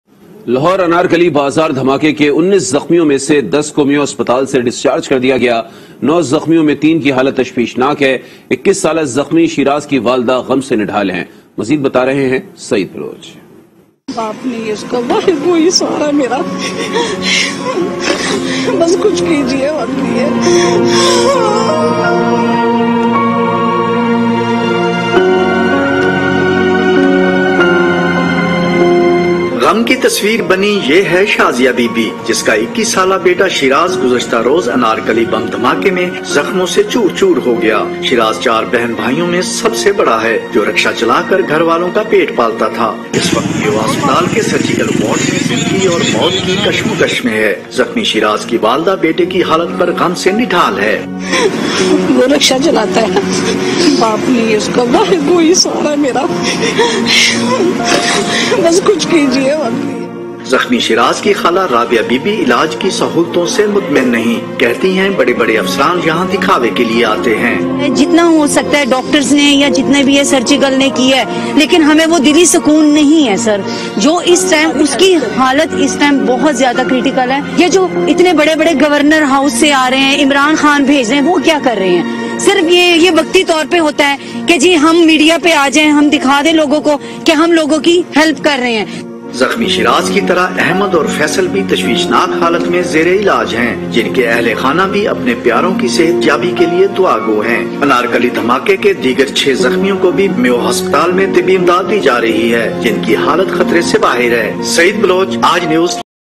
Lahore Main Dhamaka | Sheraz Ki Walda Gham Say Nidhal | Aaj News
Aaj TV Official:
shorturl.at/iBDNX